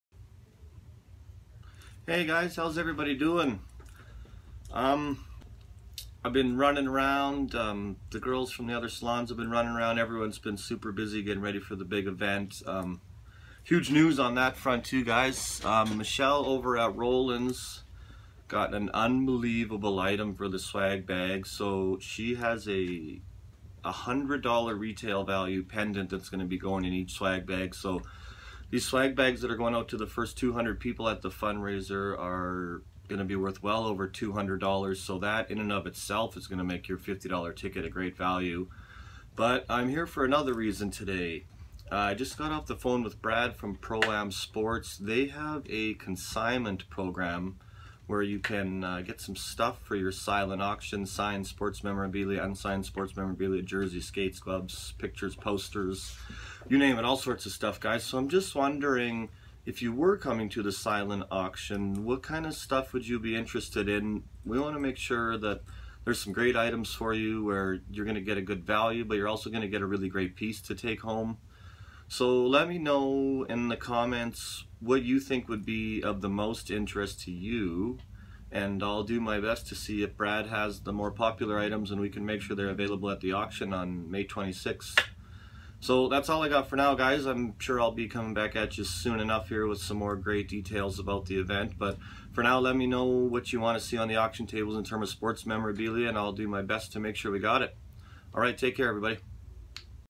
$200 swag bags
Great news about the SADA swag bags going out to the first 200 people through the doors plus I need you input on best silent auction sports memorabilia ideas.